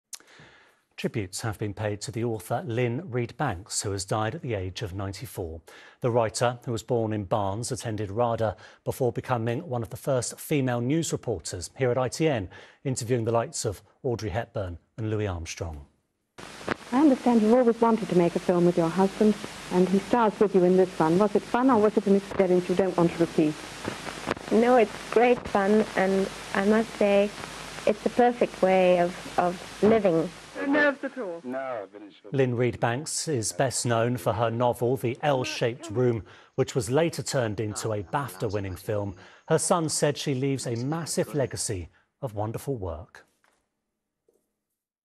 Lynne Reid Banks passes away (1929 - 2024) (UK) - UK News - 5/April/2024
Lynne Reid Banks (31 July 1929 – 4 April 2024) was a British author of books for children and adults has died aged 94.

She died yesterday. Her birthday was last July 2023.

British author Lynne Reid Banks, who wrote books including The L-Shaped Room and The Indian in the Cupboard, has died at the age of 94.

The writer died of cancer "peacefully with her family around her" on Thursday, her agent James Wills said.

She made her mark as a writer of children's literature, with her other works including Harry the Poisonous Centipede and I, Houdini.

She "leaves a massive legacy of wonderful work", said her son.

Gillon Stephenson added that he received messages daily from people telling him "what a difference she has made".

Wikipedia:
Lynne Reid Banks was a British author of books for children and adults. Banks wrote forty-eight books, including the best-selling children's novel The Indian in the Cupboard, which has sold over 15 million copies and has been successfully adapted to film.